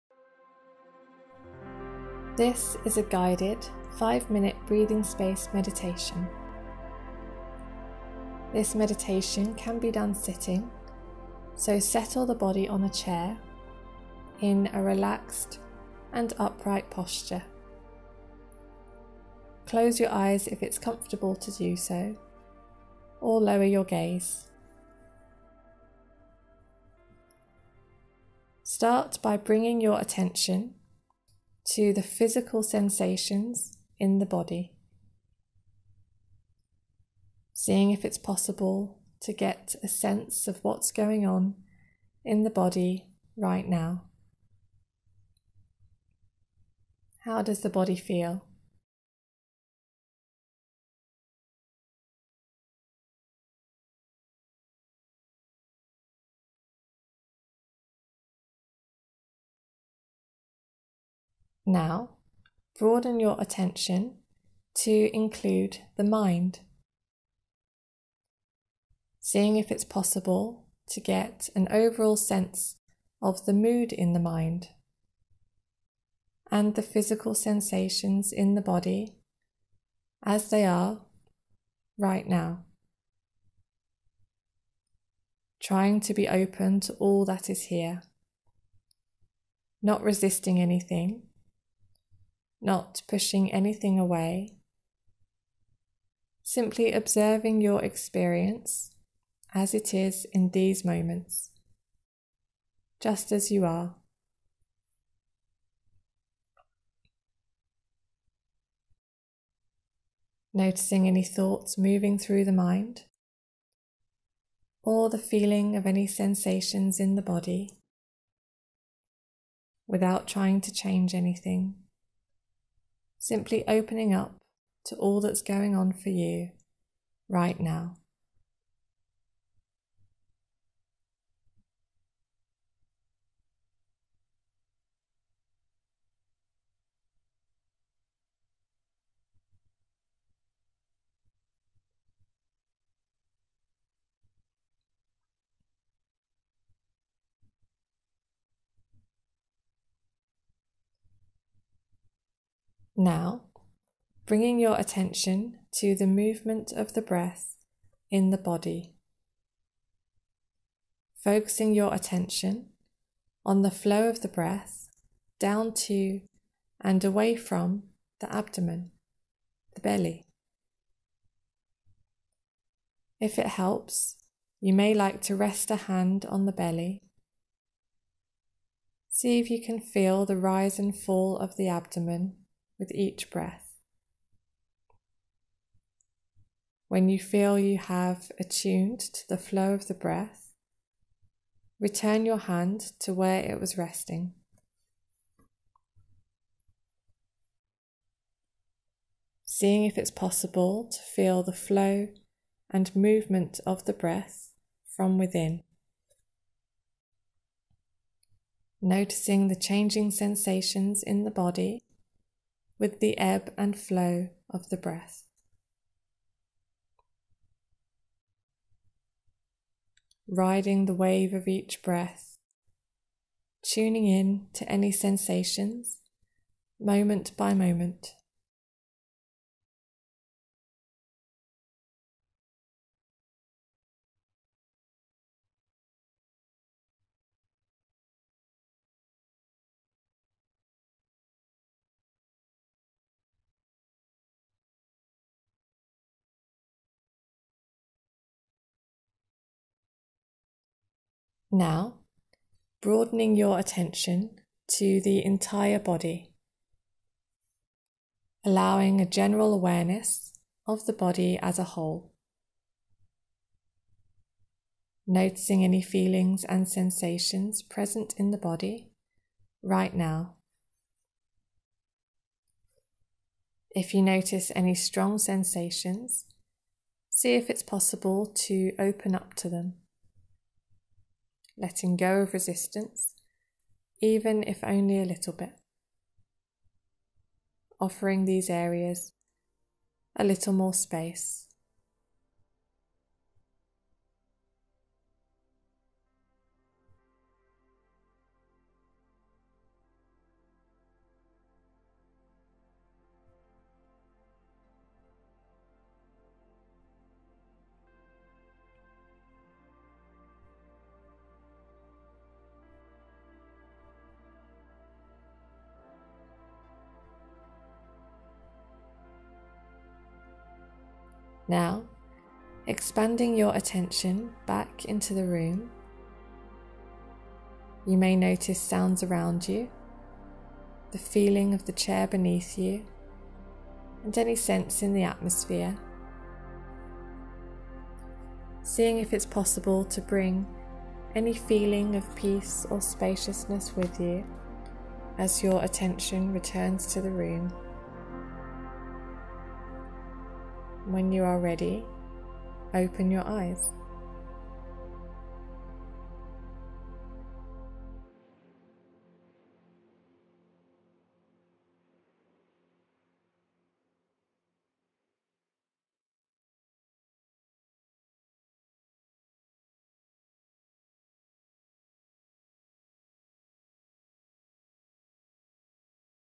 5 Minute Breathing Space Meditation helps ease back pain and stress
A quick mindfulness meditation to reset your mind and body. It can be done sitting at work or at home and will help you unwind easing back pain and relieving the stress that contributes to it. It also works for neck and should pain as well as headaches.